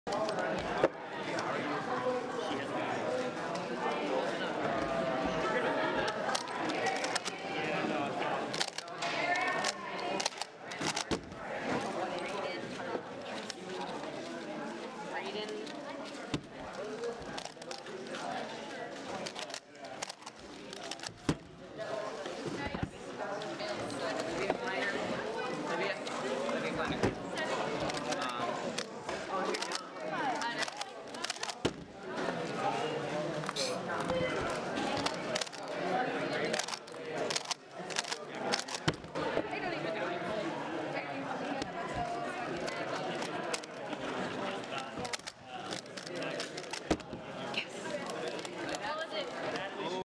4.24 Official Skewb Average [STATE RECORD]
Really glad I finally have a decent official skewb average and that I finally got first place in an event.